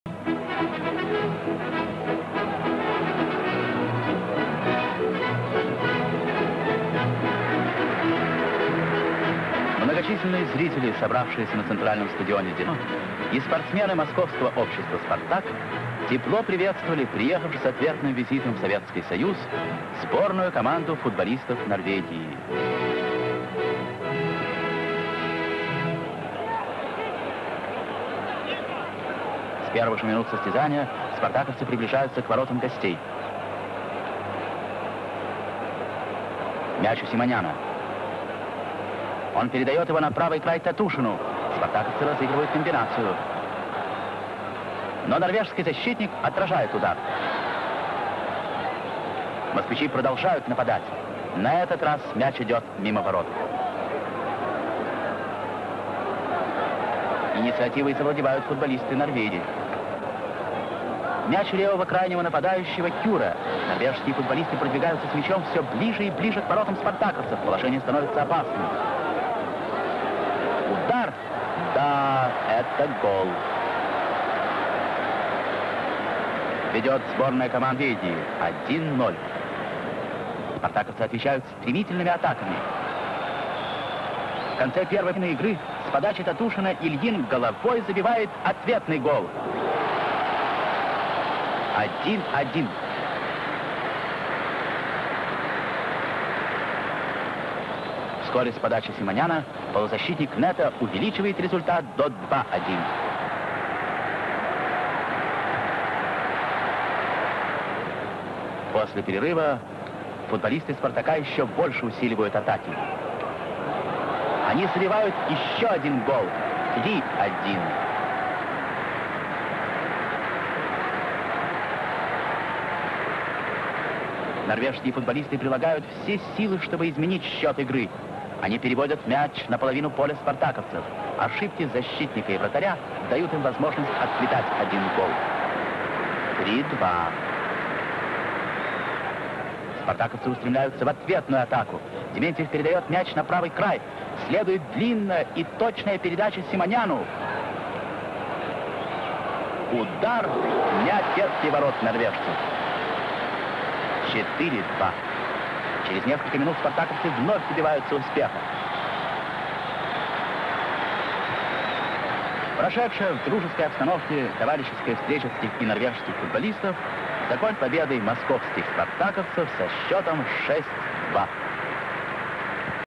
Спартак 6-2 Норвегия. Товарищеский матч 1954
Спартак (Москва) 6-2 Сборная Норвегии
Товарищеский матч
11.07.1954
Москва, стадион "Динамо"